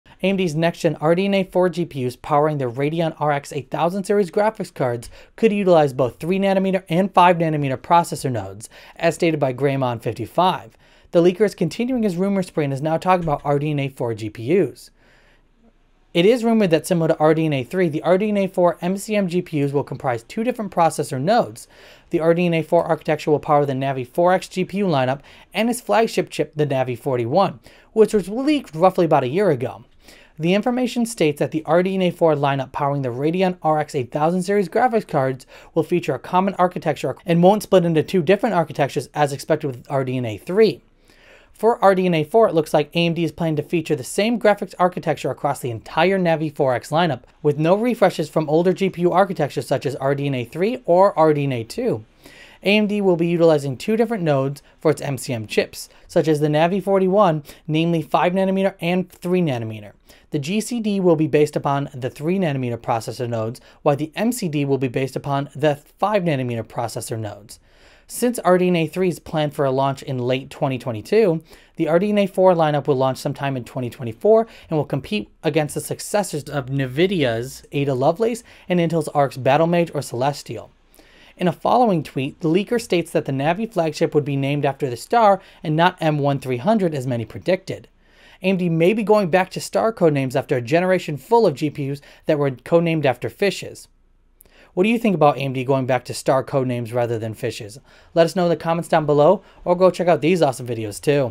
AMD's RDNA 4 GPUs Utilizing Both 3nm and 5nm Nodes?
According to Greymon55, a hardware leaker on Twitter, RDNA 4 will be utilizing both 3nm and 5nm nodes.